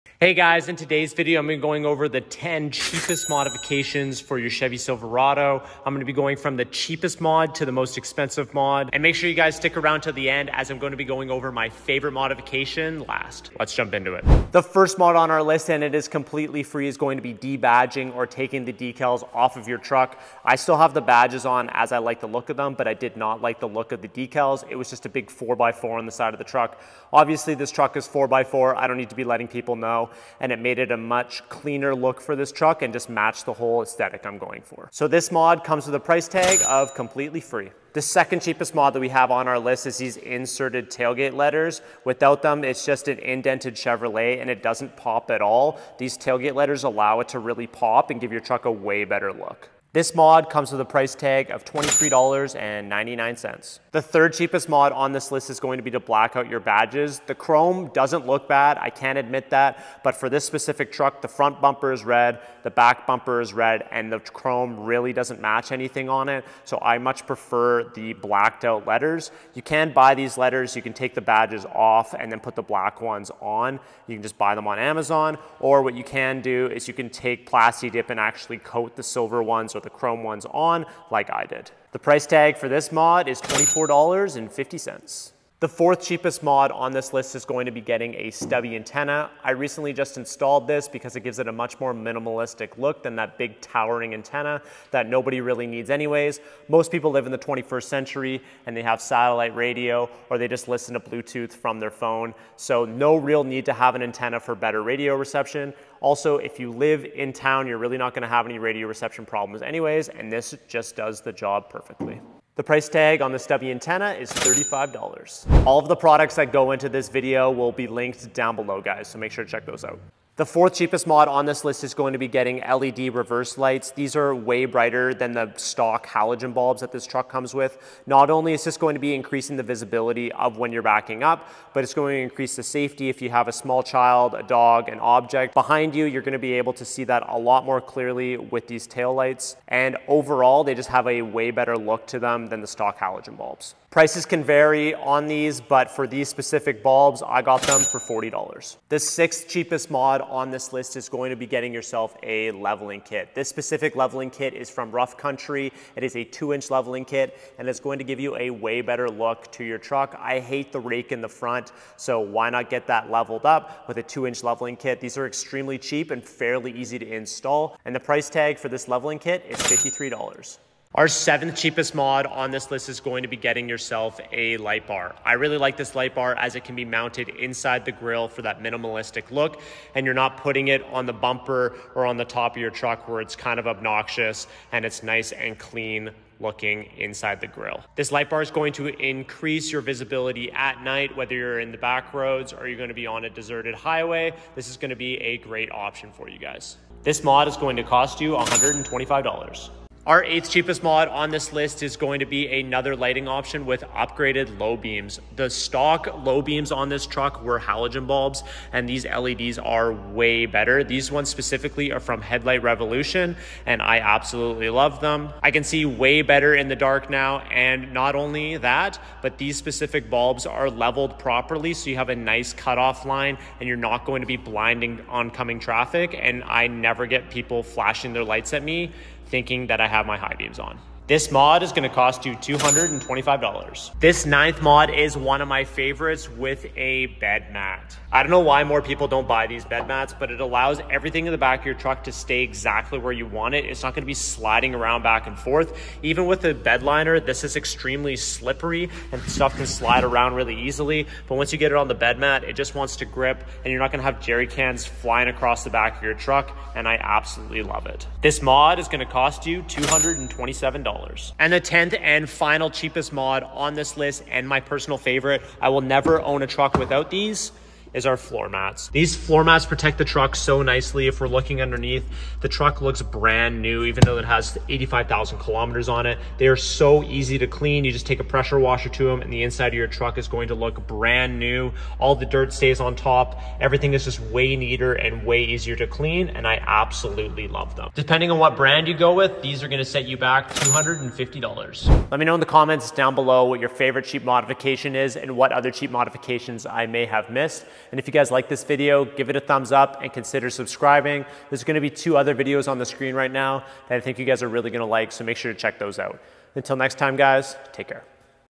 10 CHEAP Mods for your 2019+ SILVERADO
The 10 cheapest modifications for you 2019+ Chevy Silverado! Prices range from FREE to $250.

Links to cheapest mods!
2. Tailgate Letters
3. Blacked Out Badges
4. Stubby Antenna
5. LED Reverse Lights
6. Leveling Kit
7. Lightbar
8. LED Lowbeam Headlights
9. Bed Mat
10. Floormats